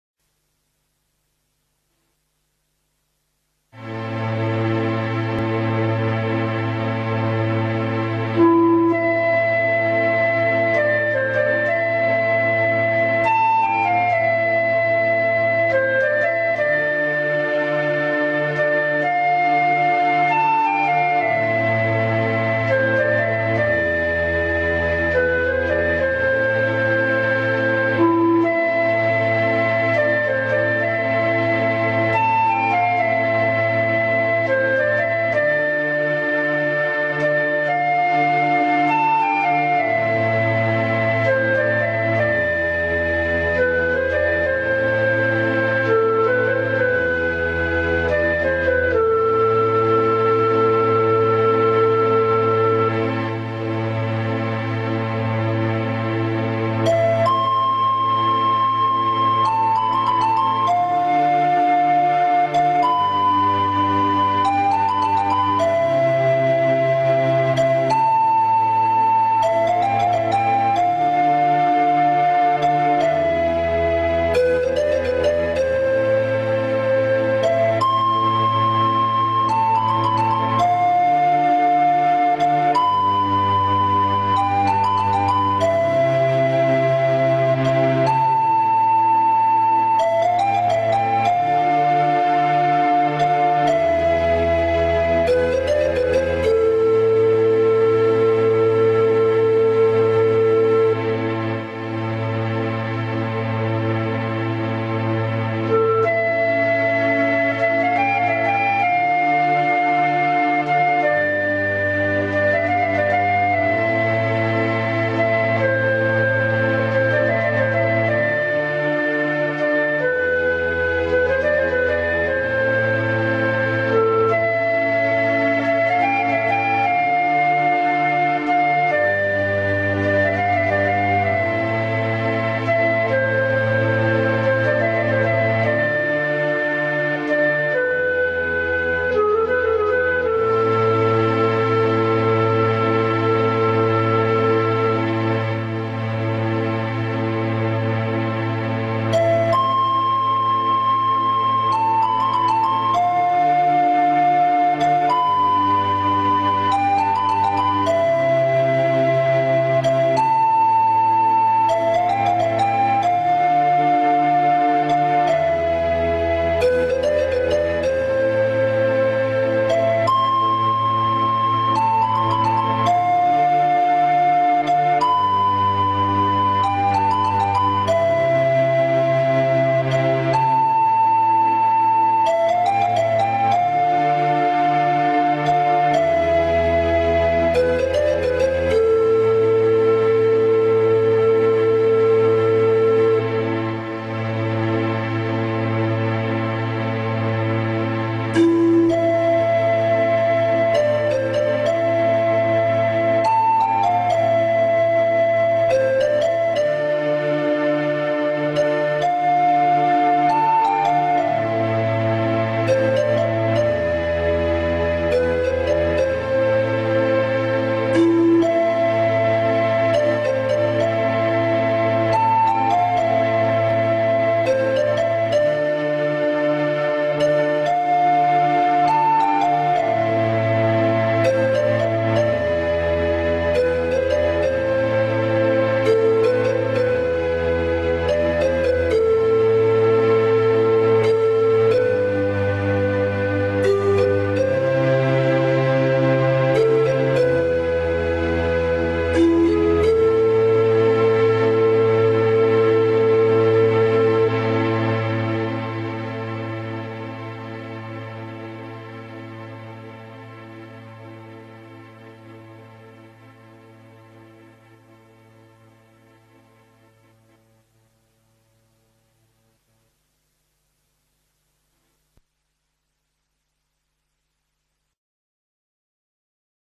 Música Maravillosa y Relajante con Paisajes impresionantes en Alta Definición. 72/877
De mi producción musical y correspondiente a la serie titulada: "Fantasías Musicales" Tema Nº 72.
Todos los temas han sido compuestos e interpretados por: Vicente Lecha Navarro.